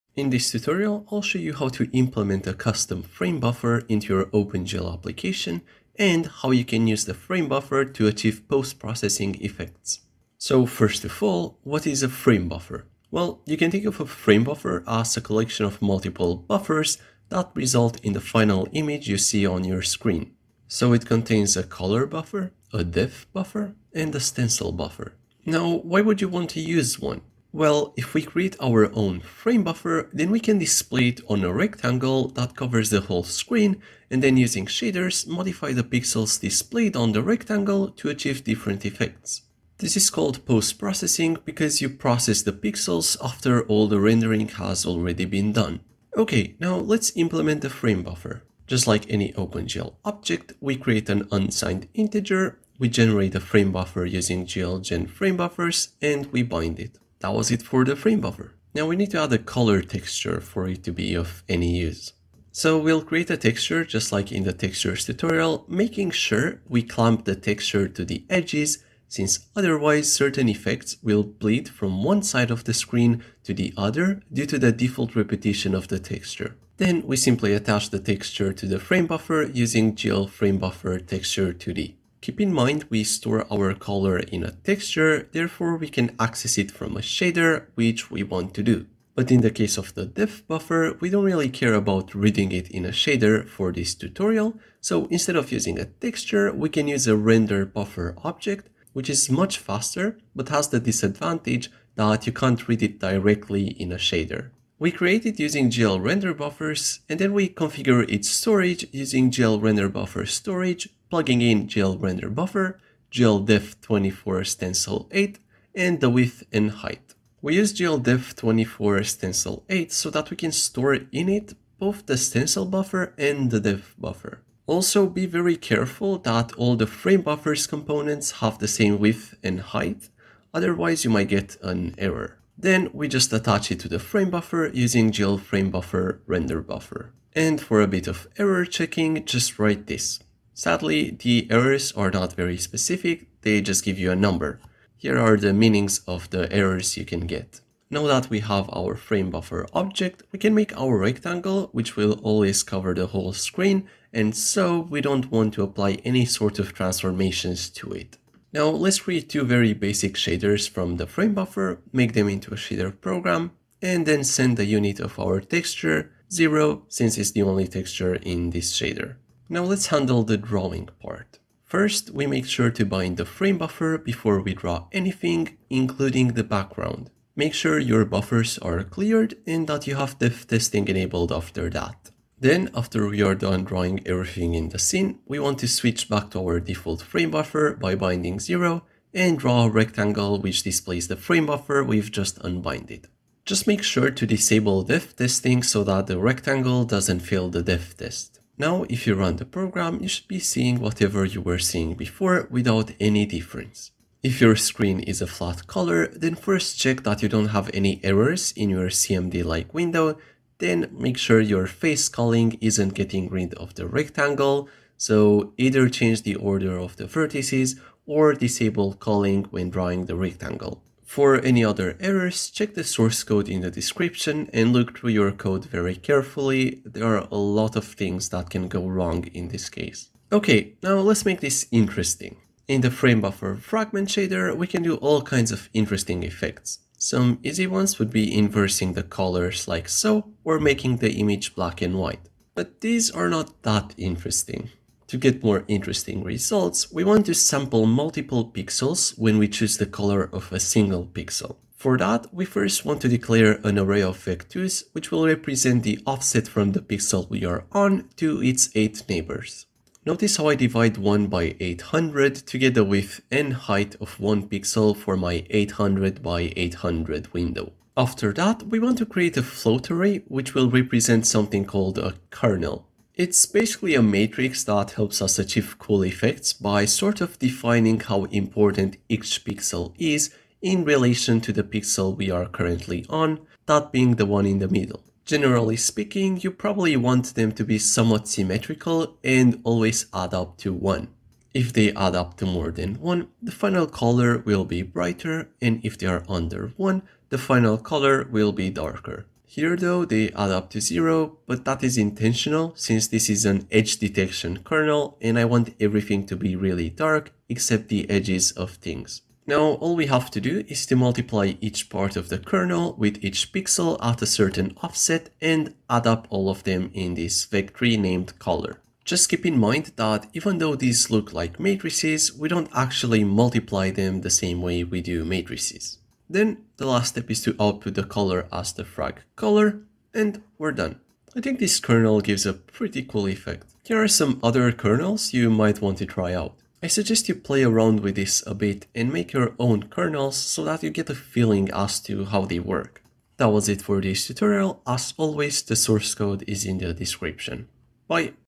OpenGL Tutorial 18 - Framebuffer & Post-processing
In this tutorial I'll show you how to create a custom framebuffer in OpenGL on which you can apply different post-processing effects.

*Timestamps*
0:00 Introduction
0:10 What is a Framebuffer?
0:24 Uses
0:46 FBO
0:59 Color Texture
1:21 Texture vs Renderbuffer
1:44 Renderbuffer
2:04 Errors
2:28 Framebuffer Rectangle
2:38 Base Shaders
2:48 Drawing the Framebuffer
3:24 Errors Tips
3:50 Simple Effects
4:05 Kernel Effects
5:36 Ending